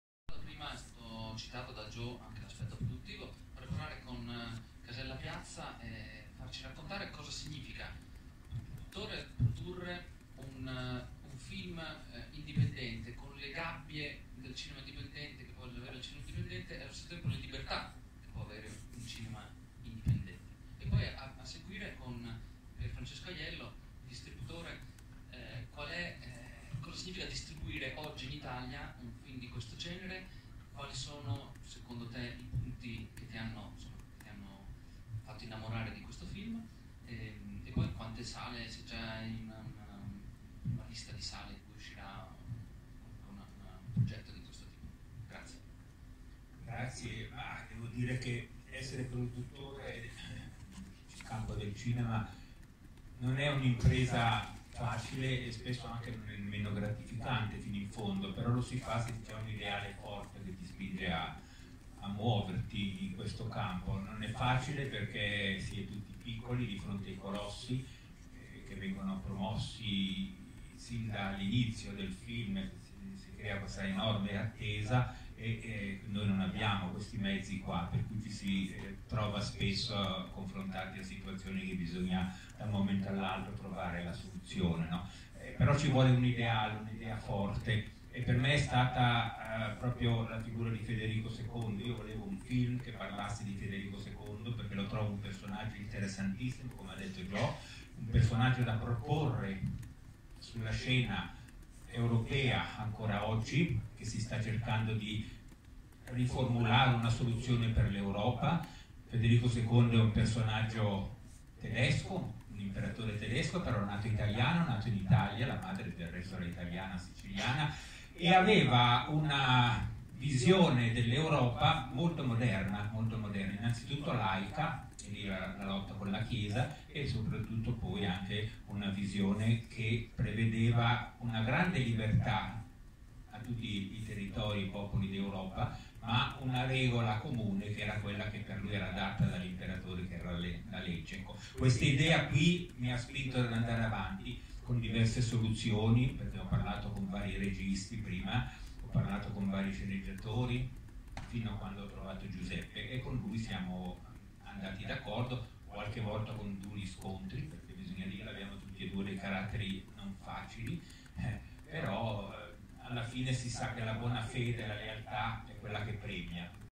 Il produttore Giovanni Casella Piazza film Oltre la Nebbia
Conferenza stampa al cinema Barberini di Roma, per il film “Oltre la nebbia” . Presenti il regista Giuseppe Varlotta, con il cast: Corinne Clery, Joe Capalbo, il produttore Giovanni Casella Piazza, il distributore Pier Francesco Aiello e l’autore delle musiche Maurizio Fiaschi.